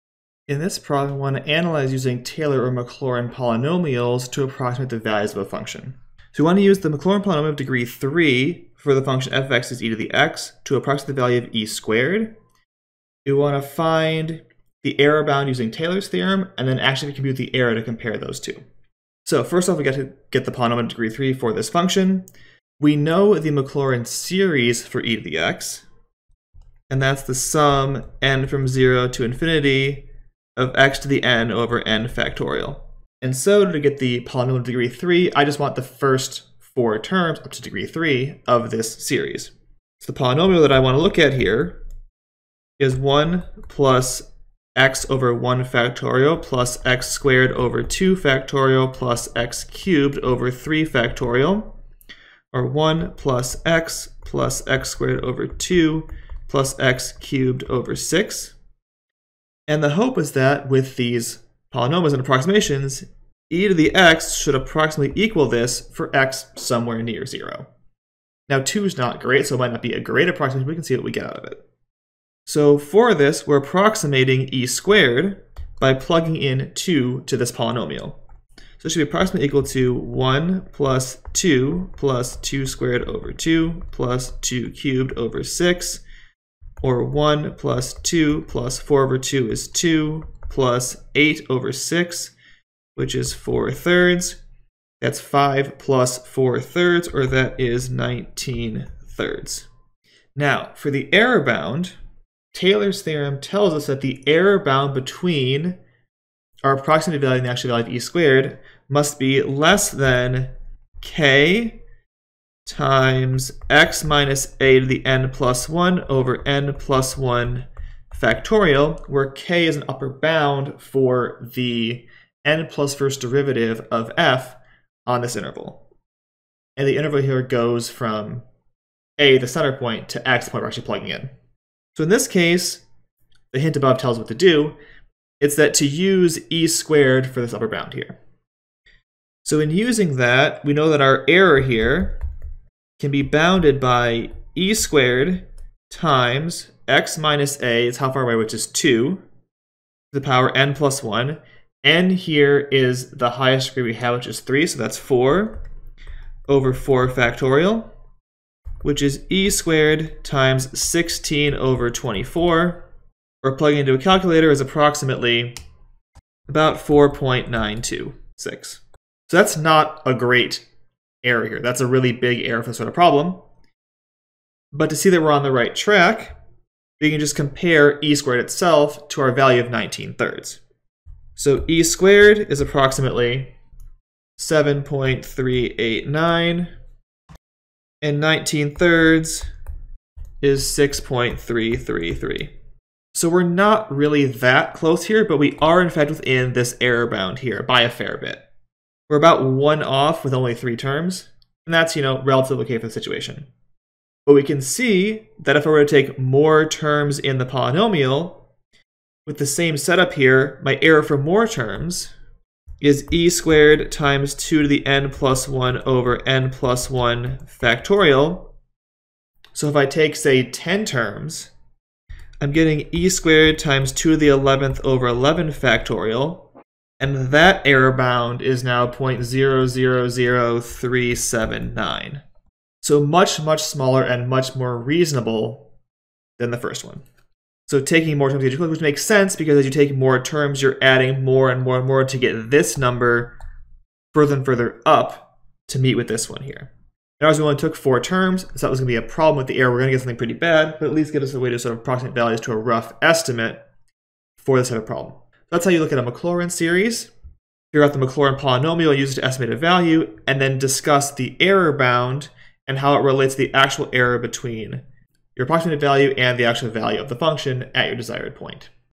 Calculus 2 - Taylor Polynomials and Error Bounds - Practice Problem
Example working out an approximation to a function using a Taylor polynomial and analyzing this approximation using error bounds.